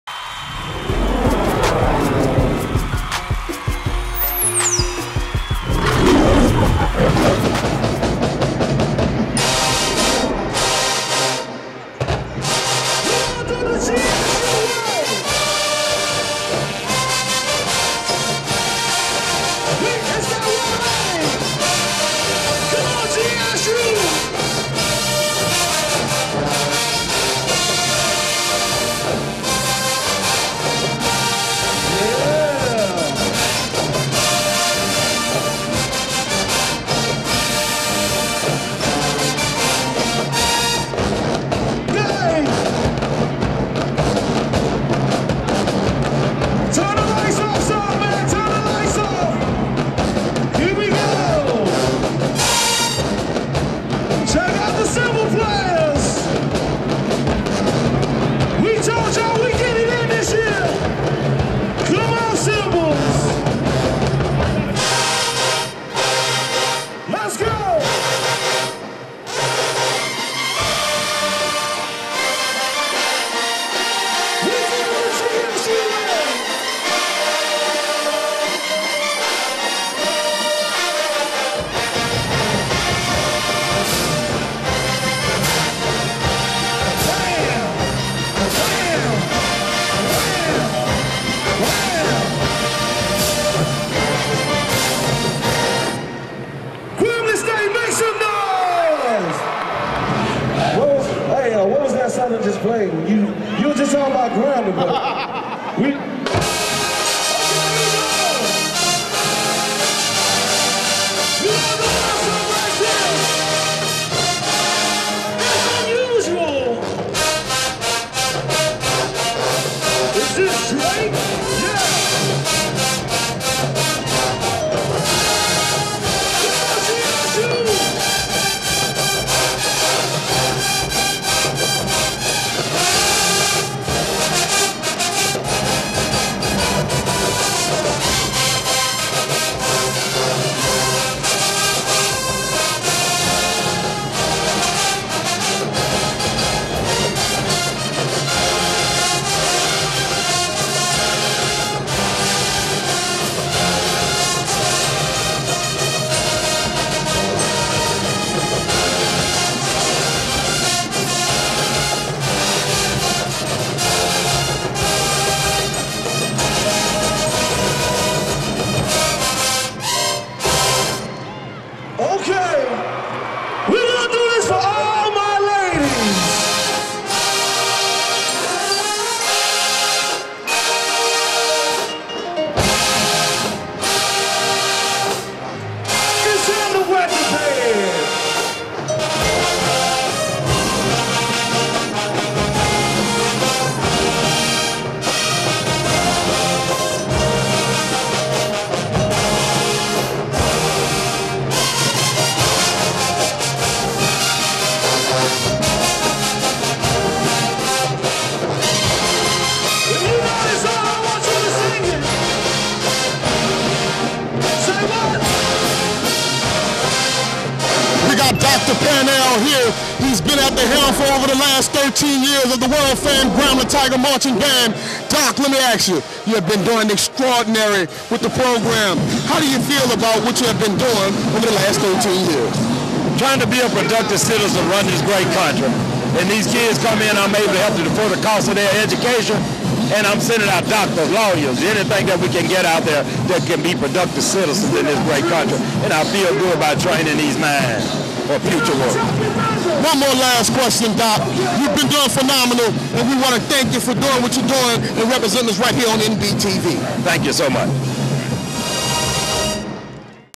Bayou Classic 2011 BOTB Grambling State Univ. Part 2
NBTV covers 2011 Bayou Classic Battle of the Bands in New Orleans.
Southern University VS Grambling State University
(Your Home for HBCU SPORTS)